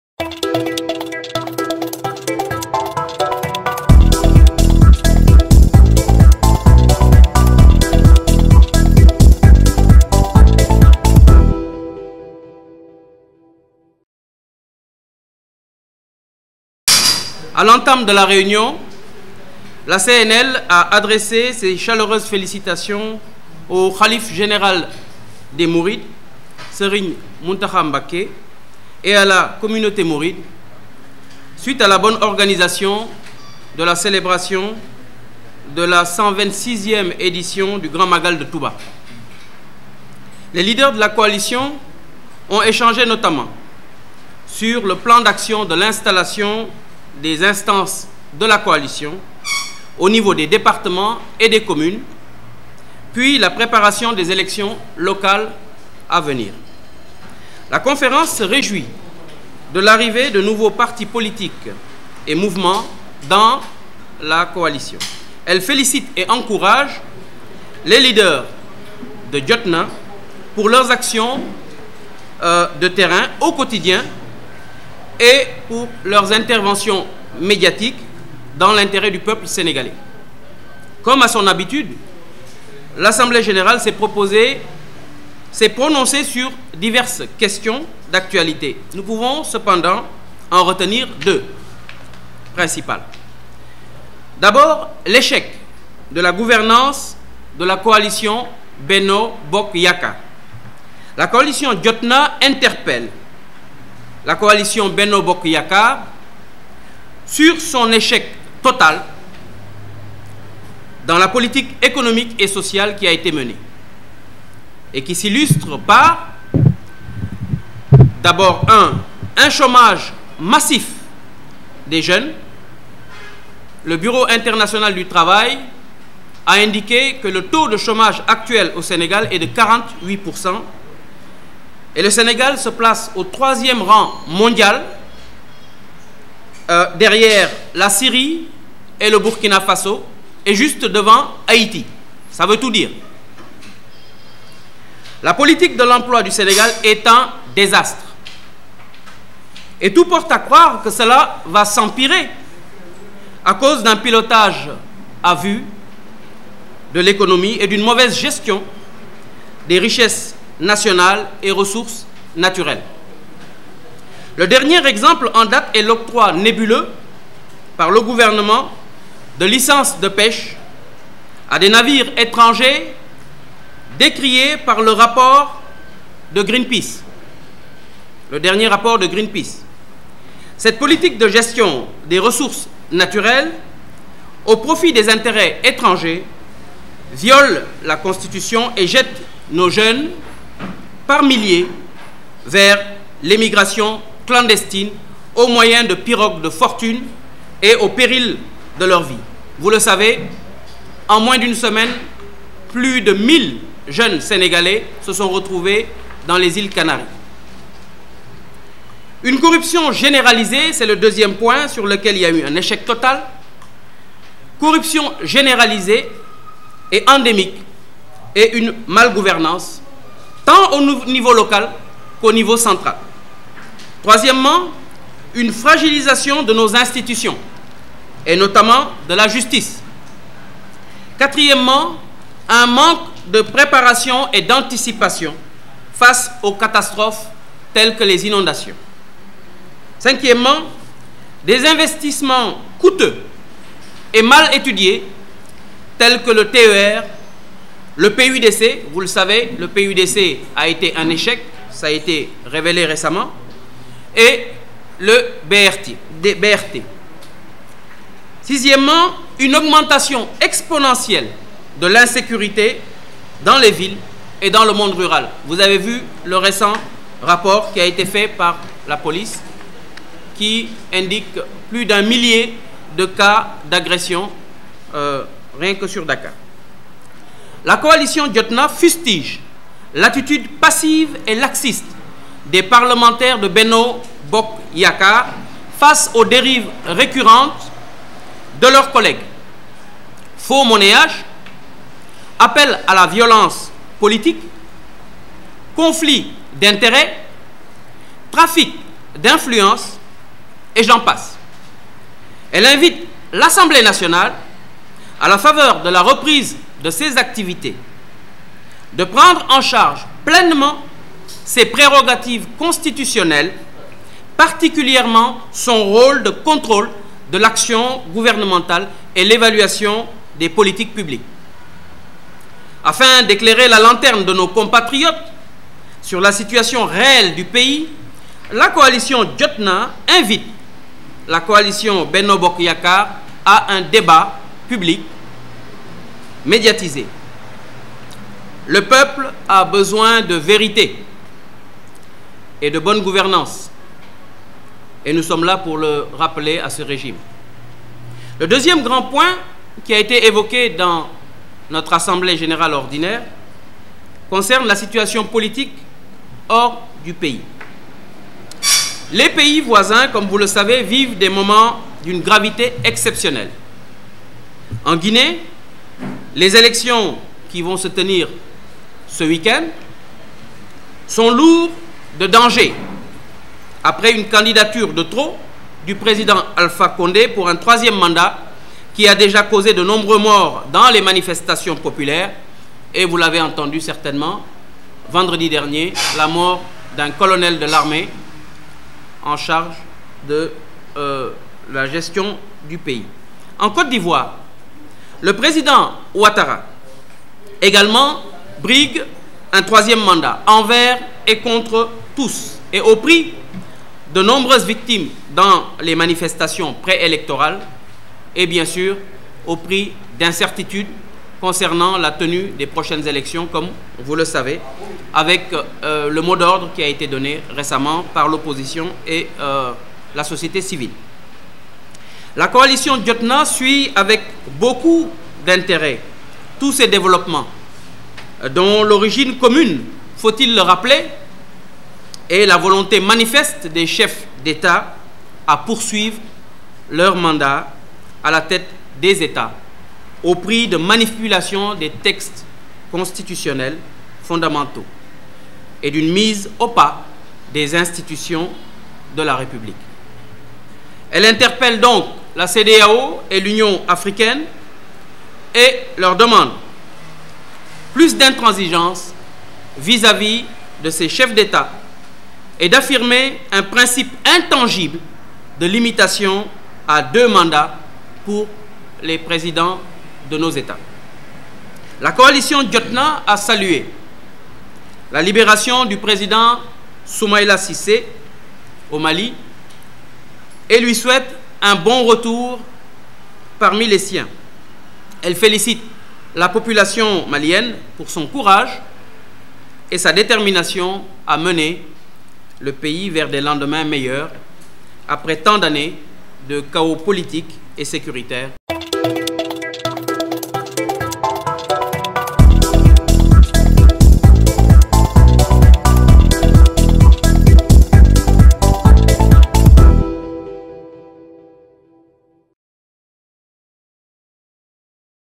CONFLIT SONKO/ MANSOUR FAYE : LA COALITION JOTNA RÉAFFIRME SA SOLIDARITÉ ET SON SOUTIEN AU........
CONFLIT SONKO/ MANSOUR FAYE : LA COALITION JOTNA RÉAFFIRME SA SOLIDARITÉ  ET SON SOUTIEN AU LEADER DE PASTEF